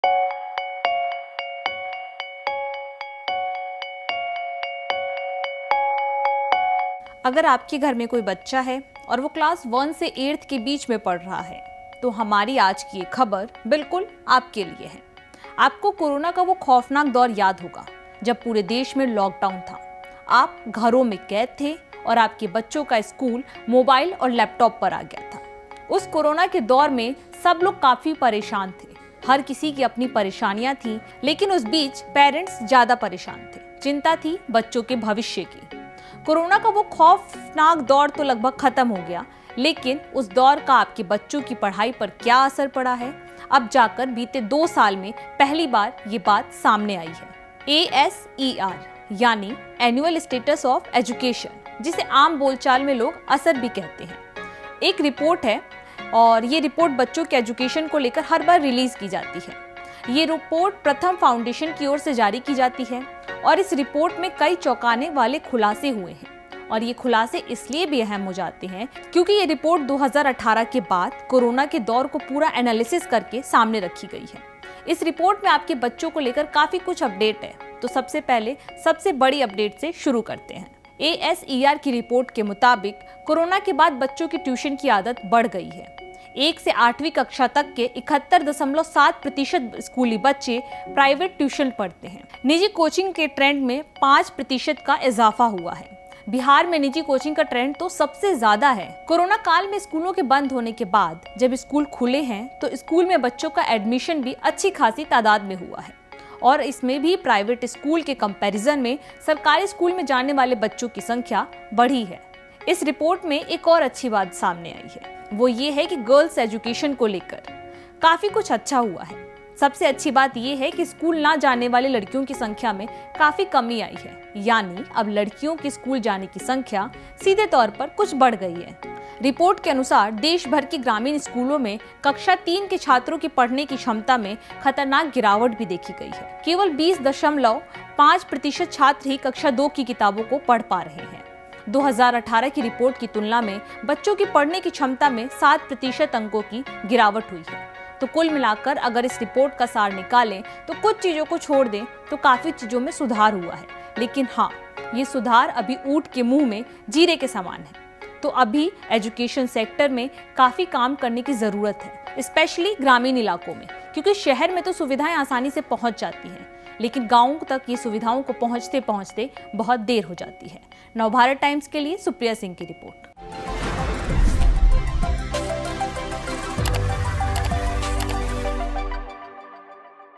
ASER Report 2022: School की जगह Tuition के भरोसे चल रही है बच्चों की पढ़ाई! Education System। NBT
Annual Status of Education Report 2022 की जारी हो गई है। Pratham Foundation की ओर से इस रिपोर्ट जारी किया गया है। कोरोना के बाद जारी की गई इस रिपोर्ट में कई खुलासे किए गए है। ASER 2022 Report के मुताबिक सरकारी स्कूल के ज्यादातर बच्चे प्राइवेट ट्यूशन ले रहे है वहीं क्लास 3 के बच्चों की पढ़ना की क्षमता की गिरावट आई है।